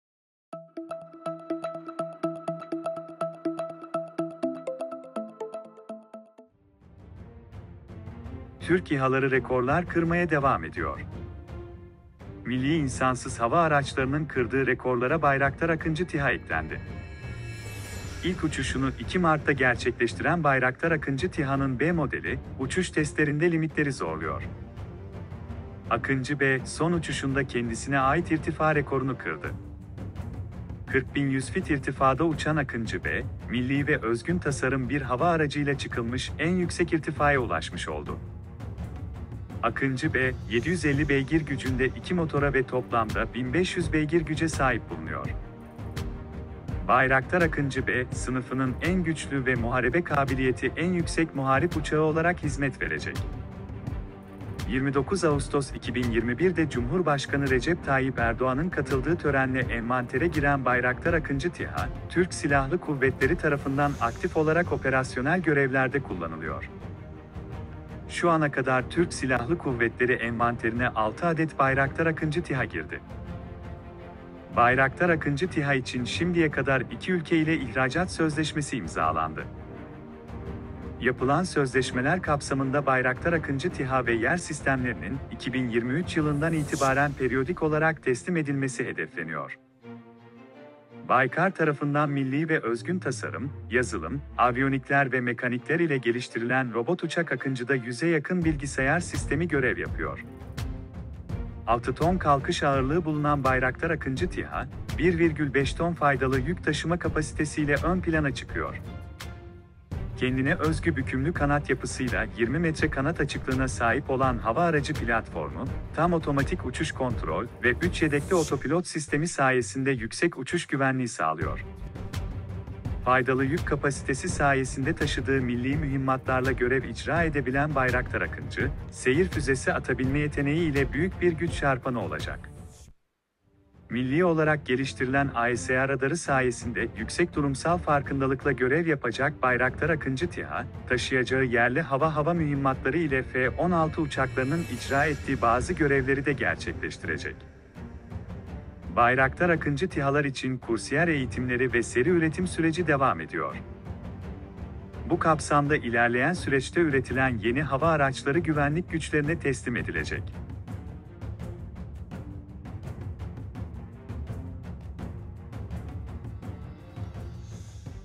Türk TİHA rekora doymuyor: Akıncı B en yüksekte - Akıncı UAV reached 40000 feet - Savunma Sanayi
Türk İHA’ları rekorlar kırmaya devam ediyor.

Milli insansız hava araçlarının kırdığı rekorlara Bayraktar Akıncı TİHA eklendi. İlk uçuşunu 2 Martta gerçekleştiren Bayraktar Akıncı TİHA’nın B modeli uçuş testlerinde limitlerini zorluyor. Akıncı B, son uçuşunda kendisine ait irtifa rekorunu kırdı. 40 bin 100 fit irtifaya çıkan Akıncı B, Milli ve özgün tasarım bir hava aracıyla çıkılmış en yüksek irtifaya ulaşmış oldu.

Türk savunma sanayi şirketi Baykar'ın ürettiği Akıncı B, 750 beygir gücünde 2 motora ve toplamda 1500 beygir güce sahip bulunuyor. Bayraktar AKINCI B, sınıfının en güçlü ve muharebe kabiliyeti en yüksek muharip uçağı olarak hizmet verecek.

Şu ana kadar Türk Silahlı Kuvvetleri envanterine 6 adet Bayraktar AKINCI TİHA girdi.

Akıncı UAV reached 40000 feet

Turkish UAVs continue to break records.

Bayraktar Akıncı UAV was added to the records broken by national unmanned aerial vehicles. Bayraktar Akıncı UAV, which made its first flight on March 2, is pushing its limits in model B flight tests. Akıncı B broke his altitude record on his last flight. Having reached an altitude of 40,100 feet, Akıncı B reached the highest altitude ever reached by a national and uniquely designed aircraft.

Akıncı B has 2 engines with 750 horsepower and a total of 1500 horsepower. Bayraktar AKINCI B will serve as the most powerful and combat-capable combat aircraft in its class.

So far, 6 Bayraktar AKINCI UAVs have entered the Turkish Armed Forces inventory.